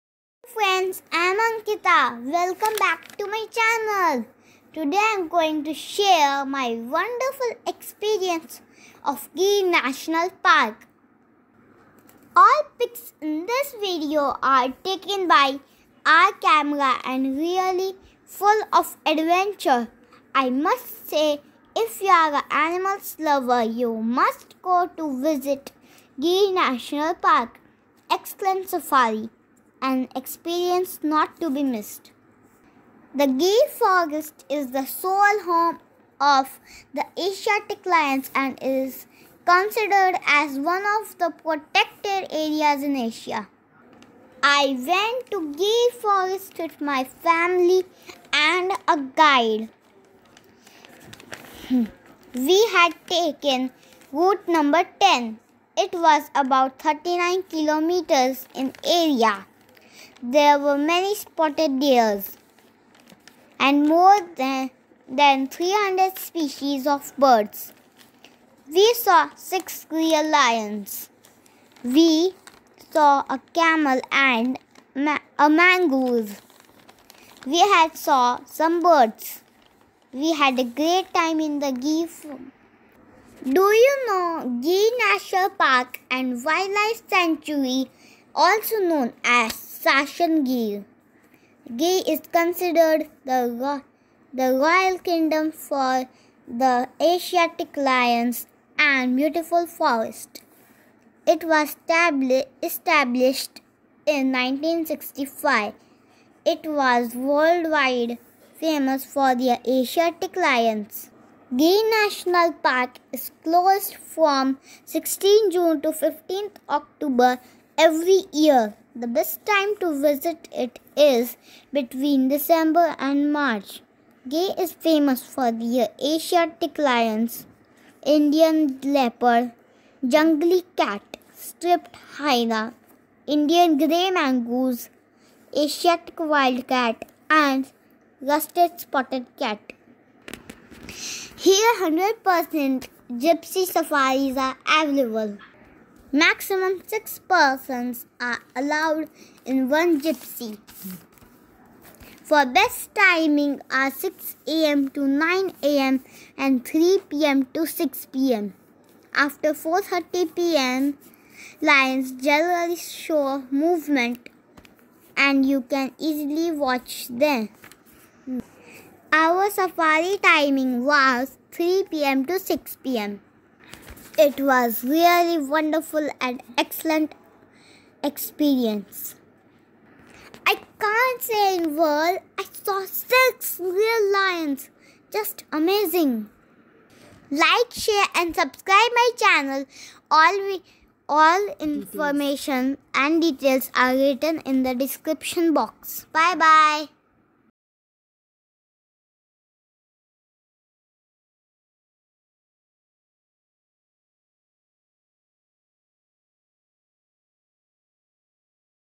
Gir National Park/Sasan Gir|Full details |Safari Timing | Sasan Gir Jungle Safari | Asiatic Lions
Best Timing for Sasan Gir Jungle Safari are 9:00am to 12 and 3:00 pm to 6:00 pm |

 Best month for visiting Gir National Park is between December and March

100% Gypsy Safari available connected by Government |
Gir jeep Safari Booking gir_jungle/safari_booking | Gir Forest is famous for the sole home of Asiatic Lions and beautiful Forest | Gir National Park and Wildlife Santurary is also known as Sasan Gir | Gir national park is worth visiting who loves nature in wild |

Distance between Somnath to Gir is 67 kms ...

Gir Safari are safe and conducted by Government | 3 hours of ride in an open jeep to explore the beautiful jungle of Gir | Maximum 6 persons and a child are allowed in one jeep 🚙..

Gir National Park is closed from 16 June to 15 October every year .

Discover wild India 🇮🇳 to visit beautiful Forest of Gir in Gujarat near Somnath(67 kms)| Indian Simba/ Asiatic Lions in Sasan Gir | jungle Safari ..

Safaris are very safe and conducted by Government with their trained and properly guided guides...